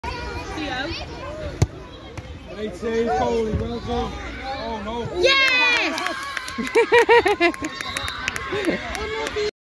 OVER HEAD KICK! WONDER GOAL! Bobby Fortune - Beechwood Barca U8’s
Freddie hits a corner and Bobby picks up on the second ball with an instinctive overhead kick to lob the keeper. Great finish! BAD FC